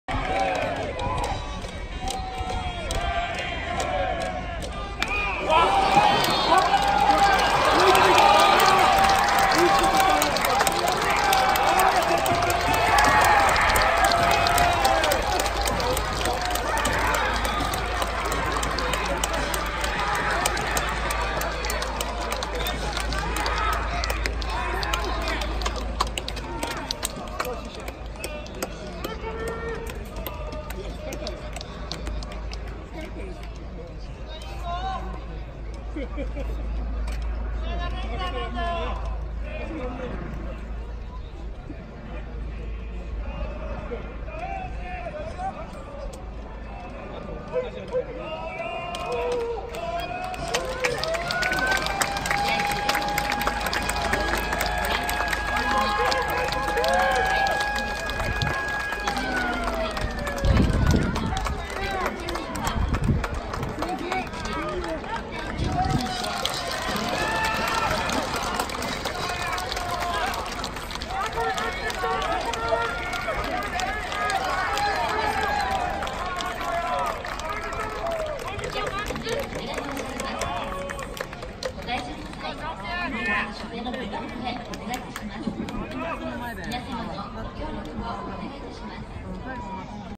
津田翔希【2019ドラフト候補】右中間へサヨナラヒット❗️東洋大学 2019年5月15日 東都大学野球 プロ注目
2019年5月15日 神宮球場 東洋大学vs立正大学